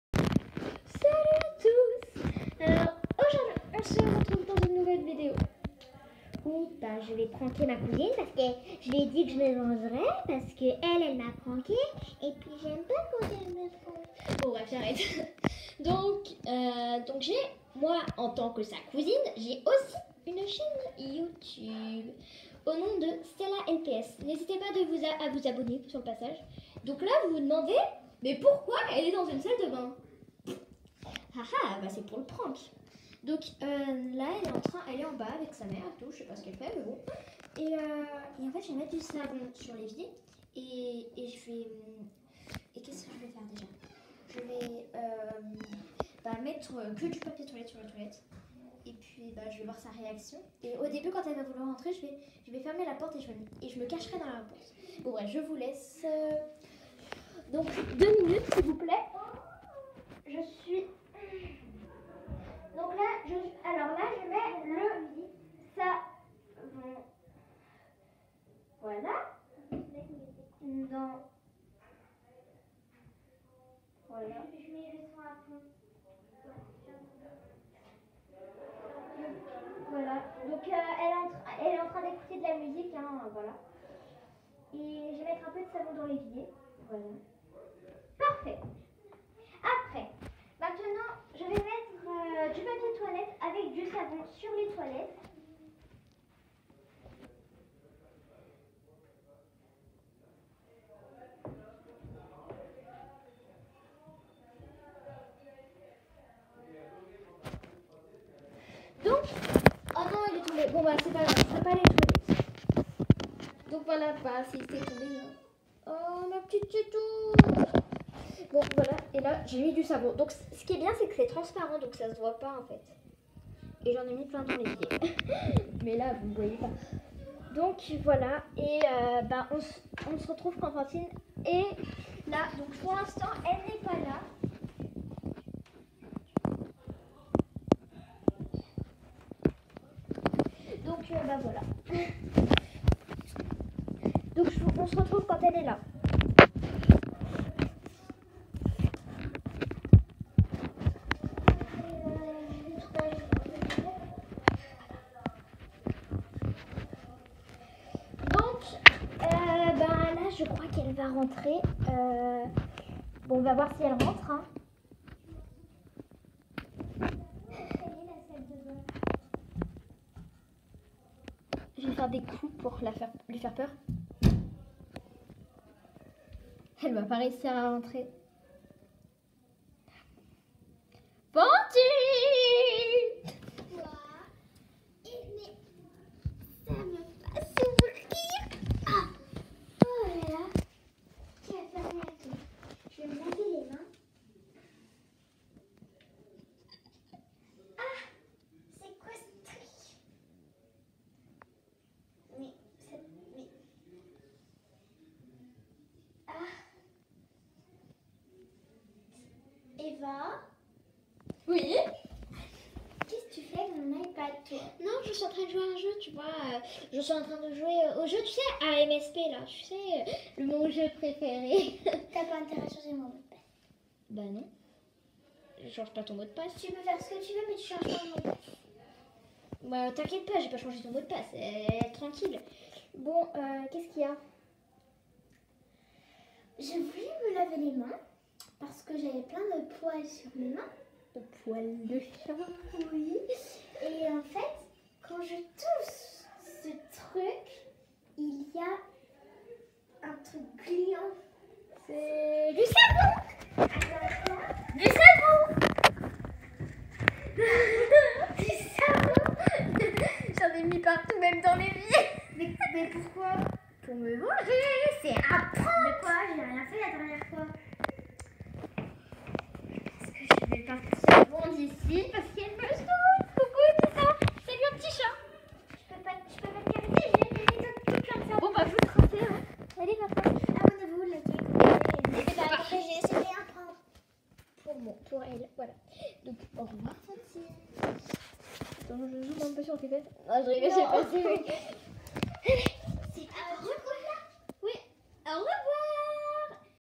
Stella LPS se venge de mon prank je pête un câble
Chaîne de ma cousine :Stella LPS